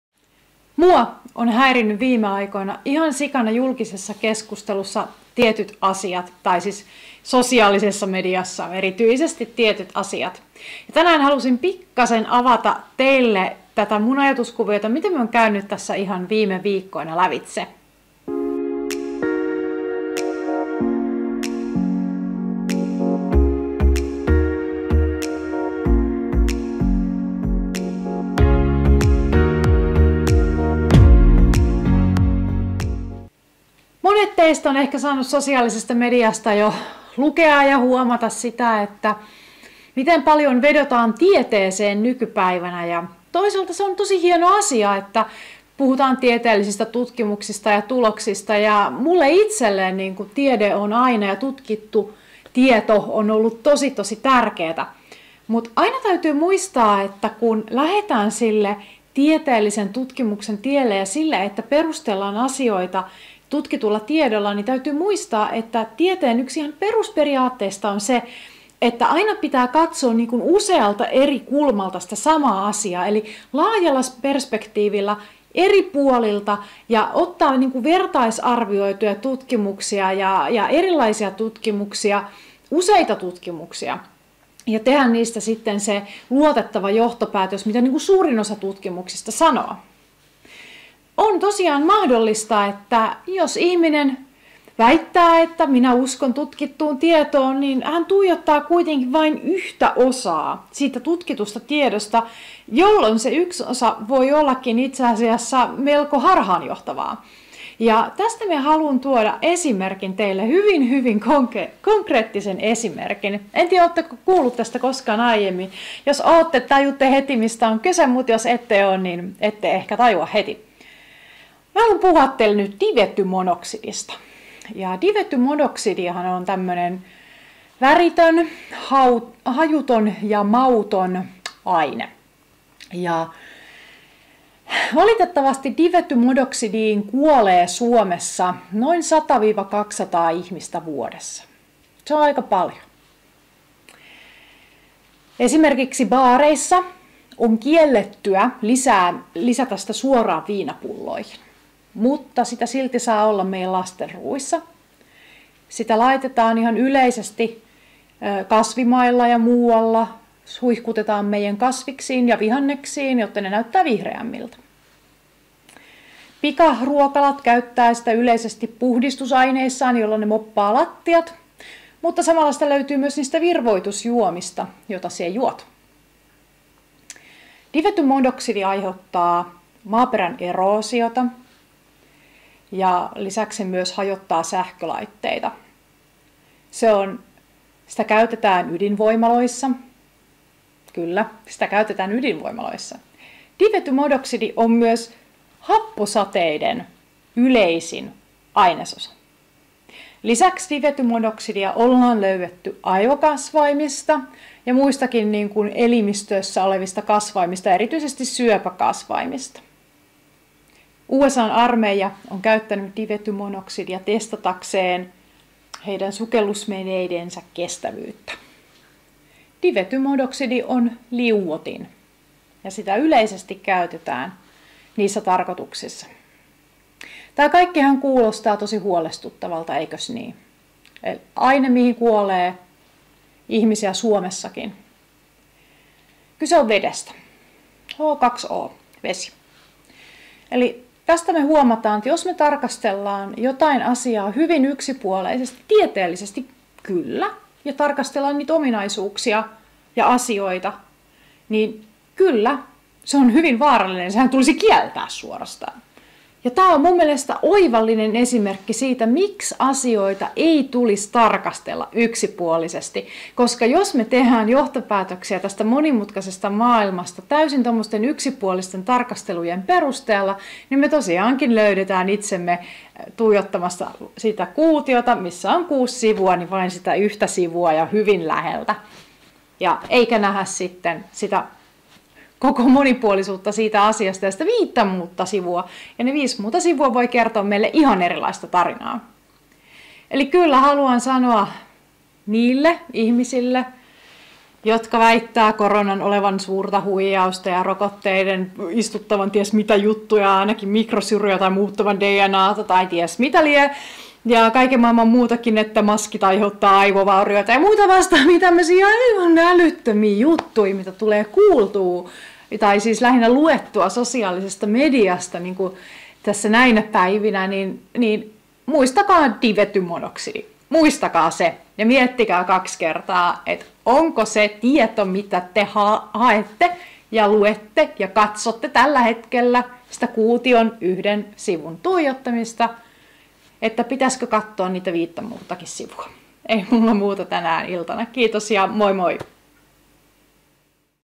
Tieteestä, tutkimuksista ja yksipuolisuuden vaaroista
Suomessa käytetään täysin yleisesti ainetta, joka aiheuttaa n. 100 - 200 ihmisen kuoleman joka vuosi. Videolla esittelen kyseisen aineen vaaroja ja lisäksi näiden esimerkkien voimin konkreettisesti osoitan, miten vaarallista on, jos nojaamme vain pieneen määrään tutkitusta tiedosta ja tieteestä, ja ohitamme suurimman osan muusta informaatiosta. Ja tämä kaikki tietenkin paketoituna koko vuoden yleisimpään puheenaiheeseen.